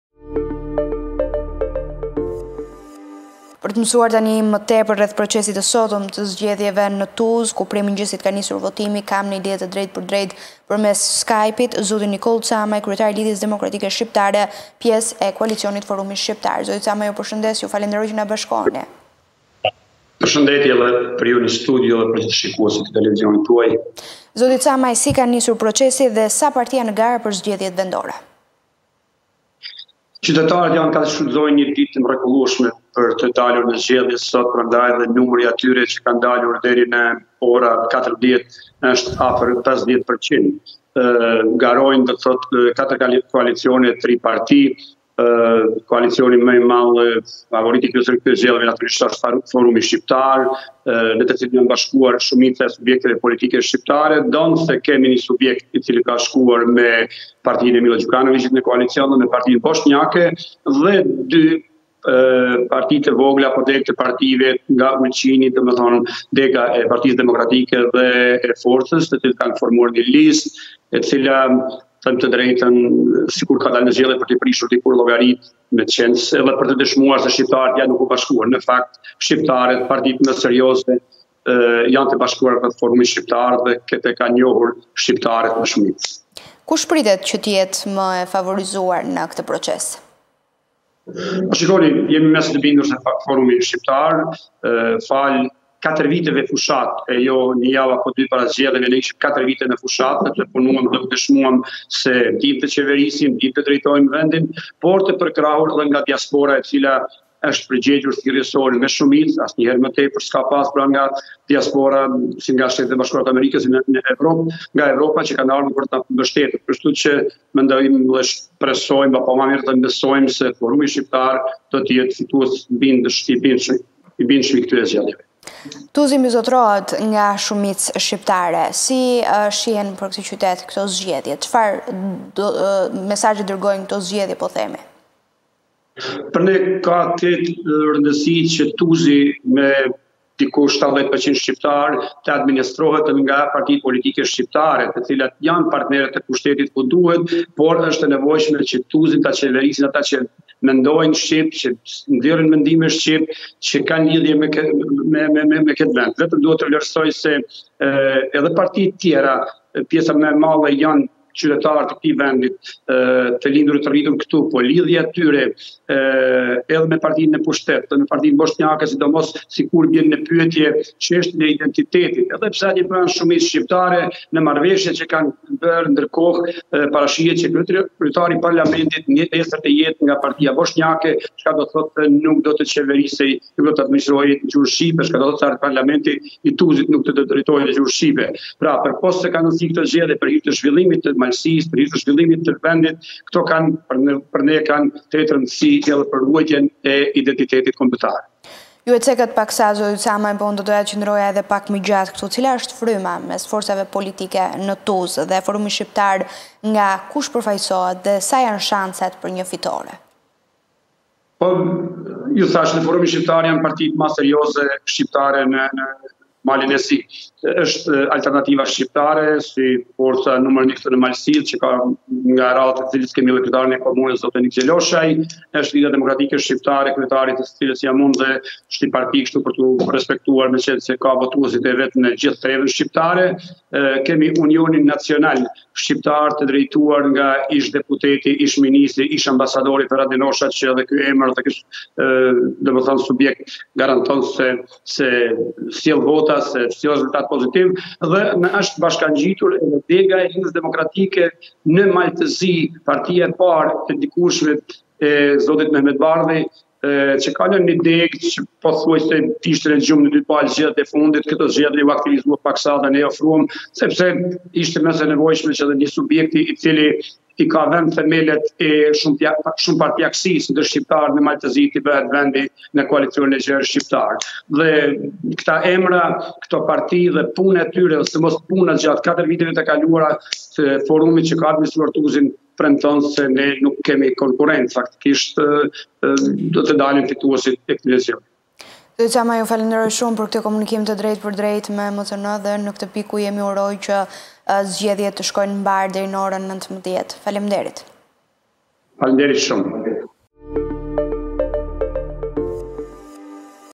Zgjedhjet vendore në Tuz, Camaj: E rëndësishme që komuna të administrohet nga parti shqiptare
Ditën e sotme në Tuz të Malit të Zi, komunë me shumicë shqiptare, mbahen zgjedhjet vendore ku në garë përballen katër koalicione dhe tri forca politike të pavarura.
Nikolla Camaj, kryetar i Lidhjes Demokratike Shqiptare deklaroi përmes një lidhje “Skype” me studion e lajmeve në MCN Tv se partitë më serioze janë bashkuar me Forumin Shqiptar, i cili do të fitojë bindshëm zgjedhjet që po zhvillohen.
Duke vijuar më tej shpjegimin e tij, Camaj tha se është e rëndësishme që Tuzi të administrohet nga parti politike shqiptare.
Sipas Camajt këto zgjedhje veç të tjerave kanë rëndësi për ruajtjen e identitetit kombëtar.